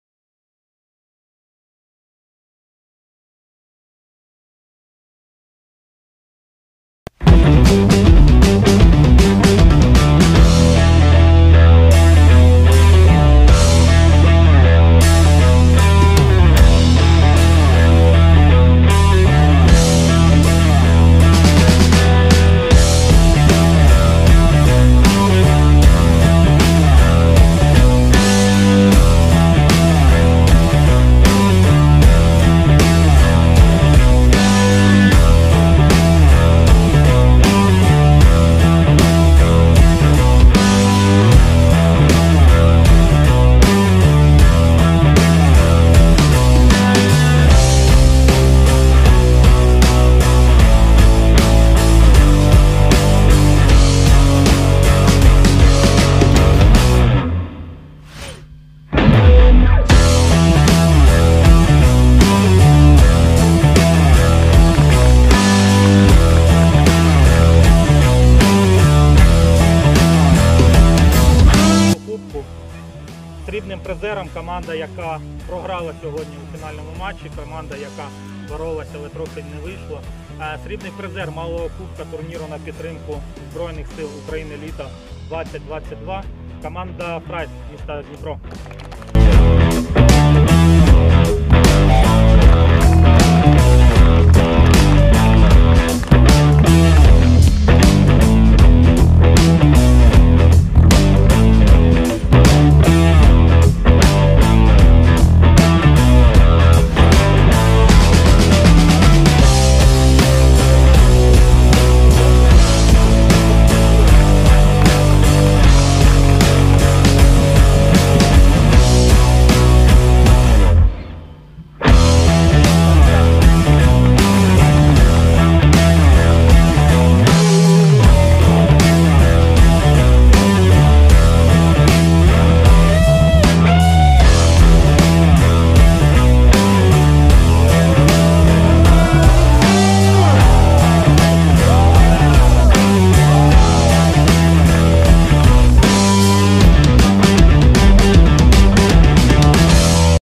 Нагородженя команд благодійного турніру на підтримку ЗСУ «Літо 2022»
10 вересня пройшла фінальна частина та нагородженя у благодійному футбольному турнірі серед аматорських команд під назвою «Літо 2022», який проходив на підтримку Збройних Сил України.

Починаючи з серпня, у змаганнях приймали участь одразу 16 колективів, які були спочатку розподілені на 4 підгрупи, а згодом розбиті на дві частини, а саме на Малий та Великий кубки. Було дуже символічно, що фіналістів змагань сьогодні привітав та нагородив Олімпійський чемпіон з футболу, наша легенда - Олексій Чередник разом із Виконавчим директором АФДО - Ярославом Дерев‘янко. Отже маємо такі результати:

Малий Кубок🏆:
1. ФК «СтальДМЗ» (м. Дніпро)
2. «FRAIT» (м. Дніпро)

Великий Кубок🏆:
1. ФК «Енергія-Тополь» (м. Дніпро)
2. ФК «AM-Estate» (м. Дніпро)
3. ФК «Оріль» (Царичанський район)

Нажаль матч за 3-е місце між командами «СКМД» та «МДЮСШ» на 28-ій хвилині було перервано через повітряну тривогу. Переможець цього матчу буде визначений наступного тижня.

Долучаємось до привітань всіх учасників турніру і бажаємо, щоб наступний літній турнір відбувся вже під мирним Українським небом🇺🇦!